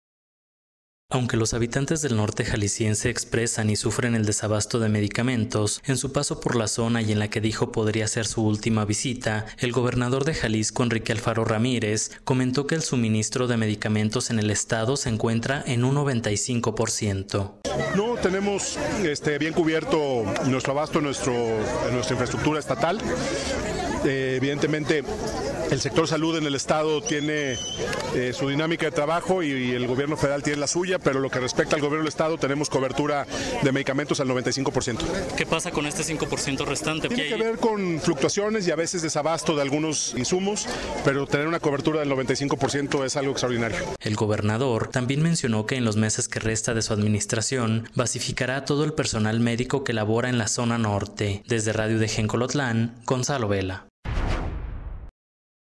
Alfaro asegura un 95% de cobertura en abasto de medicamentos
Aunque los habitantes del norte jalisciense expresan y sufren el desabasto de medicamentos. En su paso por la zona y en la que dijo podría ser su última visita, el Gobernador de Jalisco, Enrique Alfaro comentó que el suministro de medicamentos en el estado se encuentra en un 95 %.